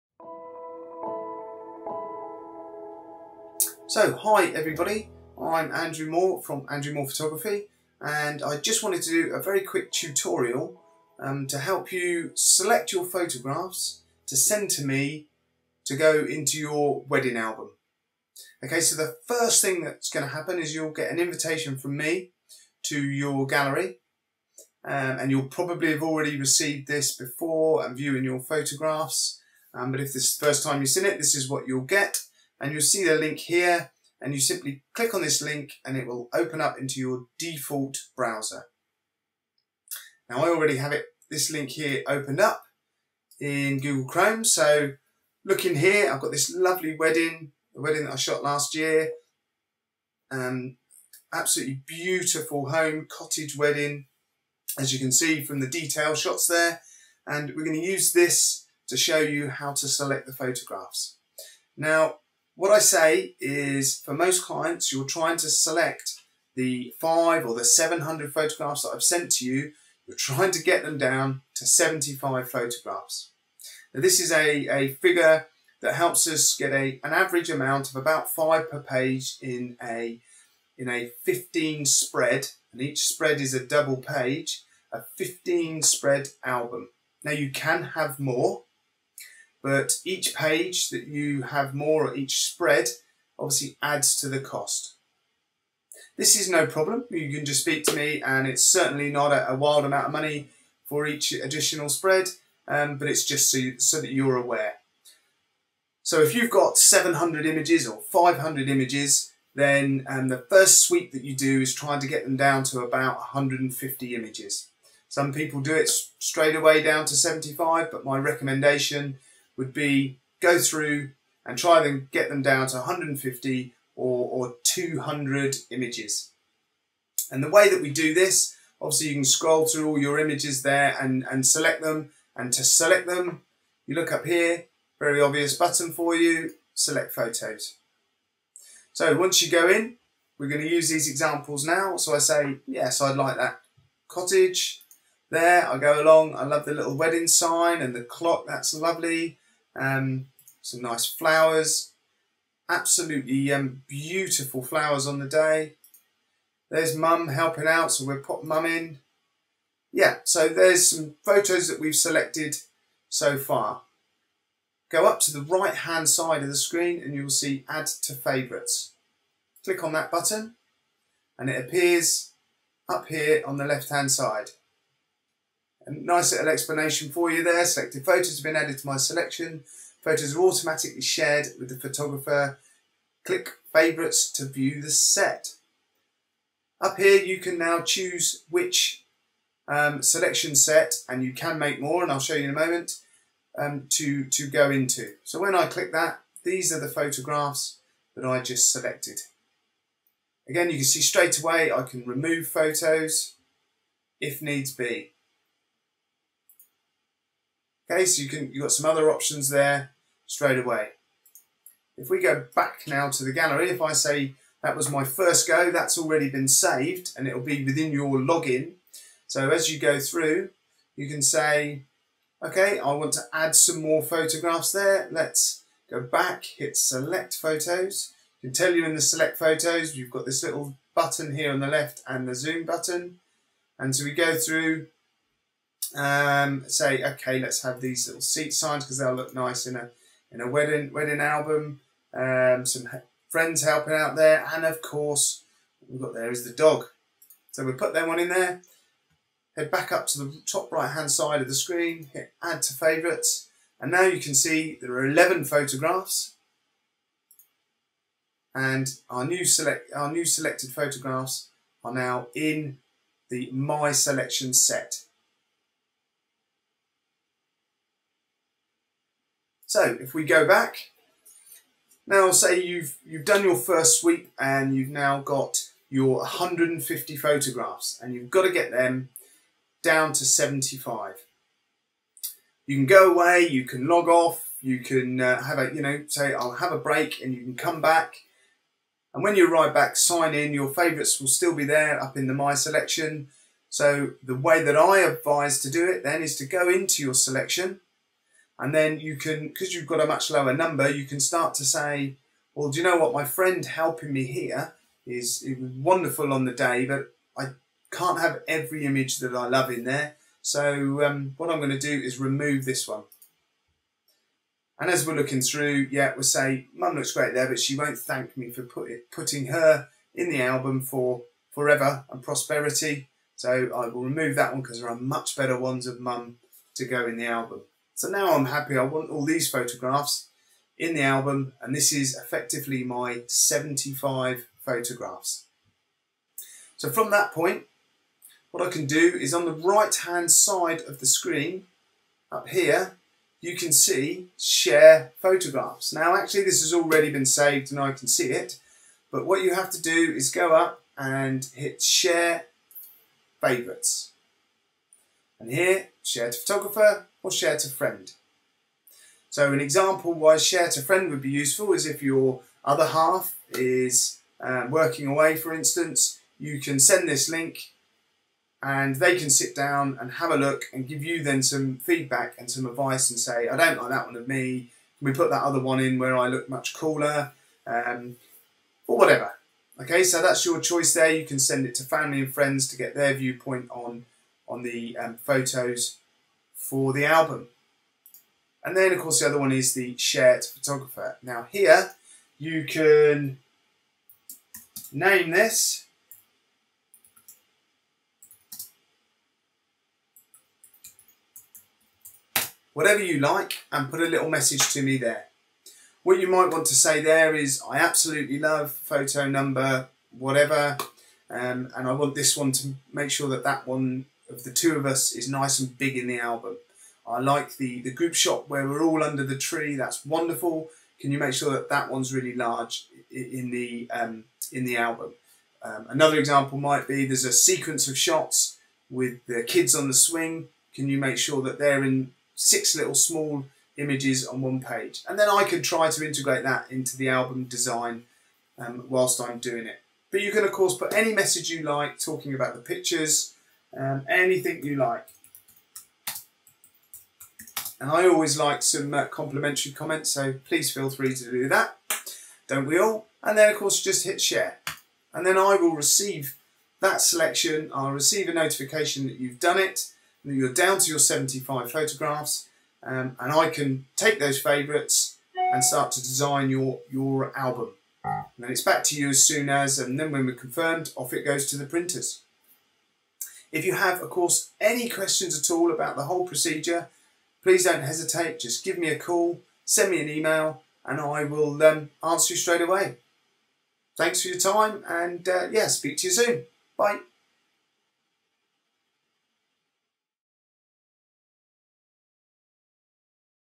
Selecting photos for album for Andrew Moore Photography in Zenfolio gallery
Selecting photos for a wedding album using Zenfolio gallery favourites